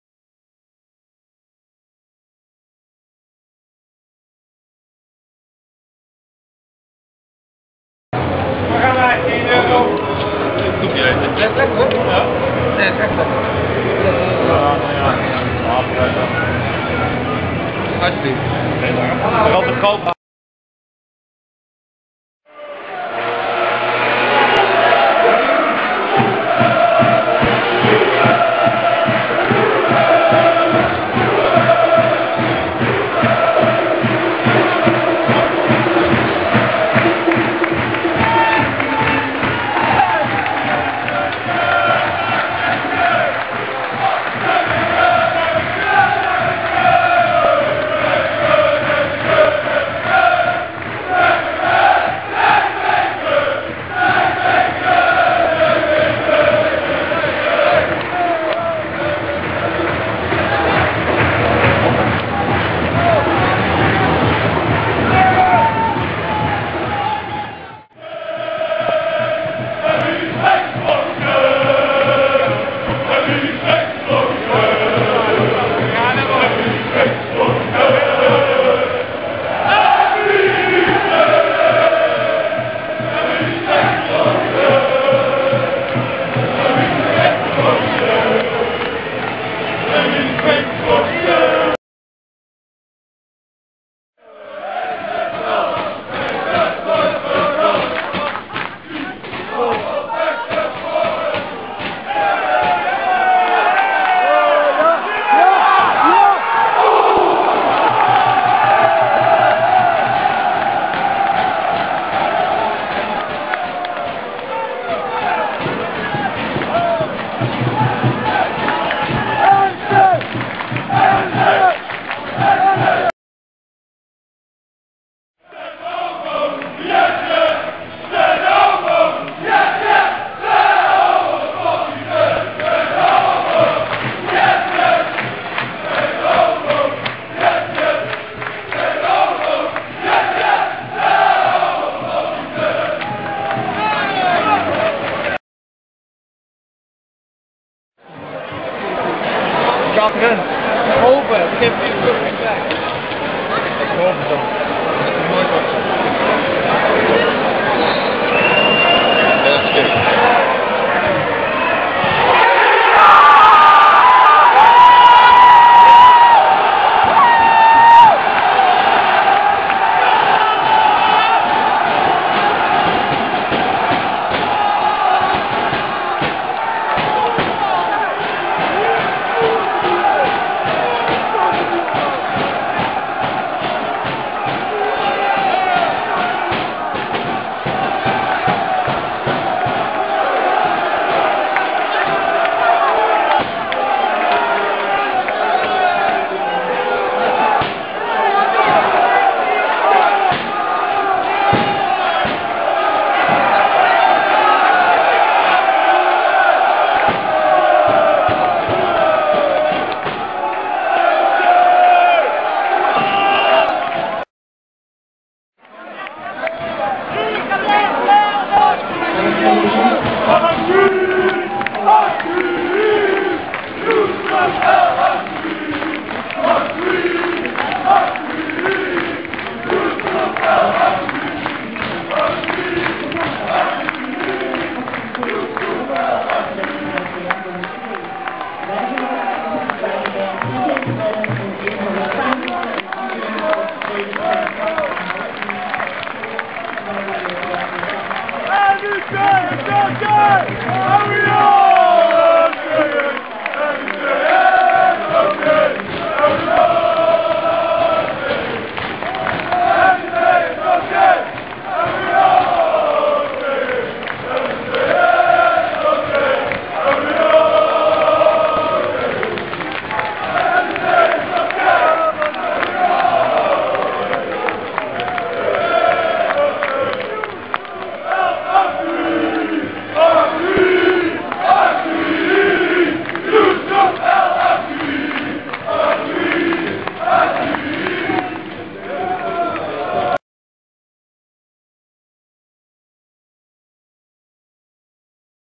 De Graafschap-NEC sfeerimpressie
Sfeerimpressie van De Graafschap-NEC, uiteraard vanuit ons uitvak!